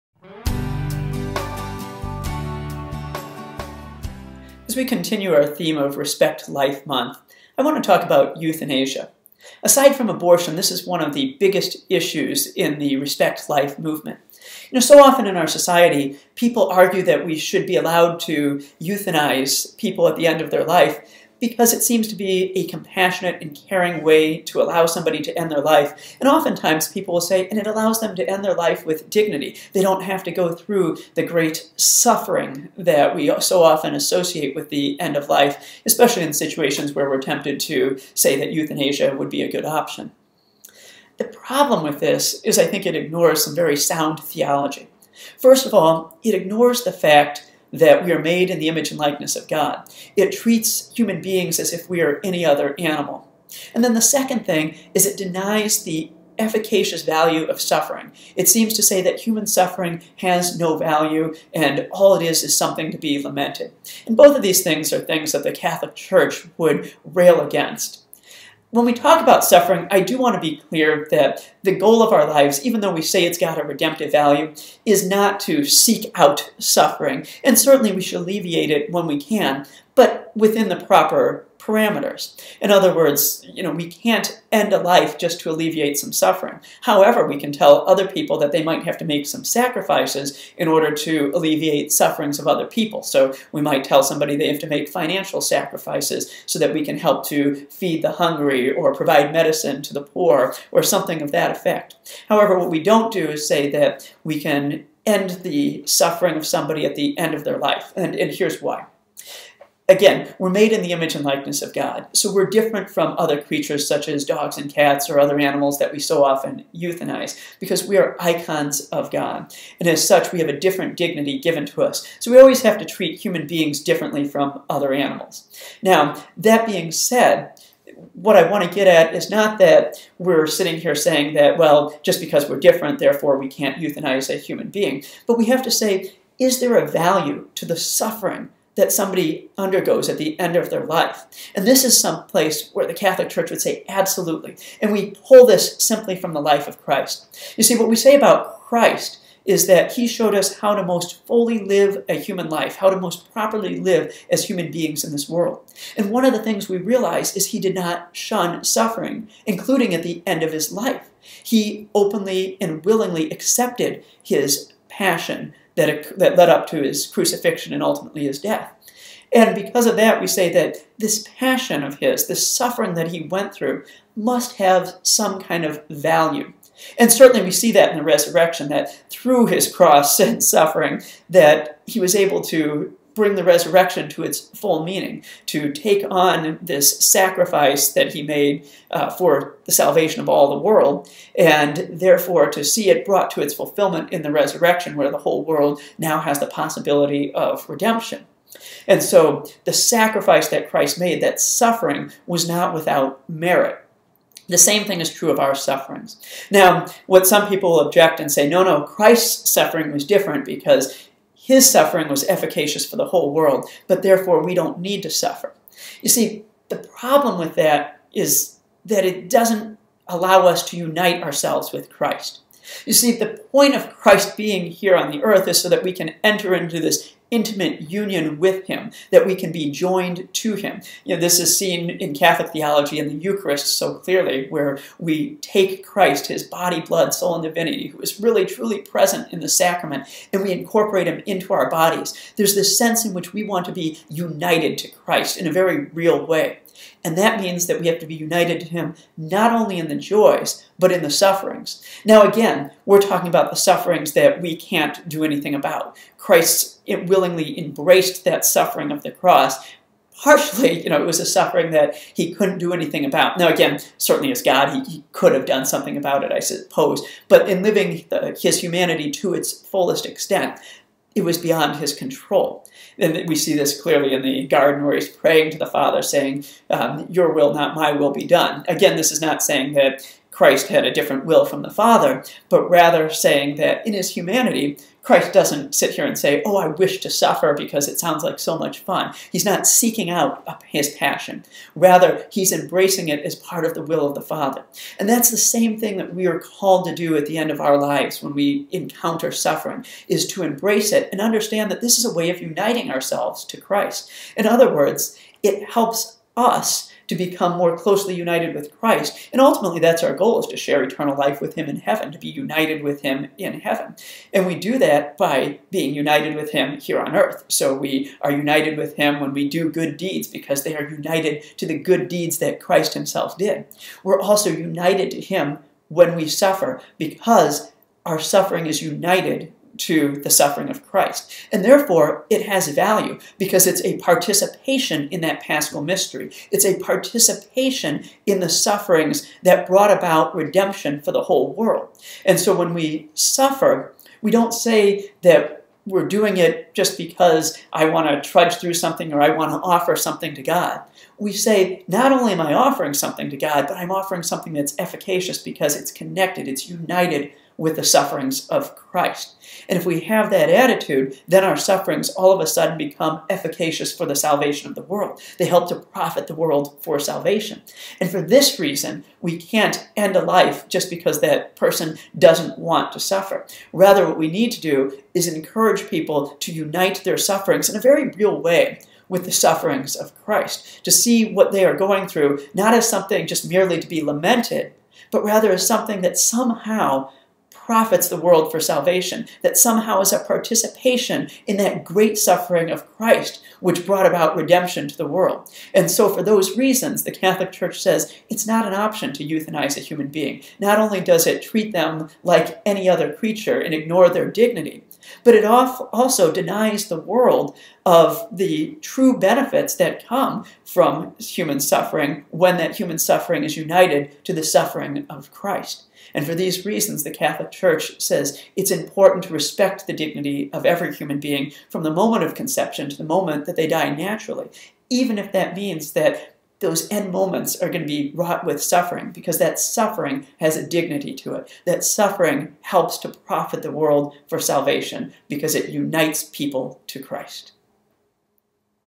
Respect Life - Euthanasia
October is Respect Life Month. Fr. Brian speaks to the issue of Euthanasia in regards to Catholic teaching about the dignity of human beings and the value of human suffering.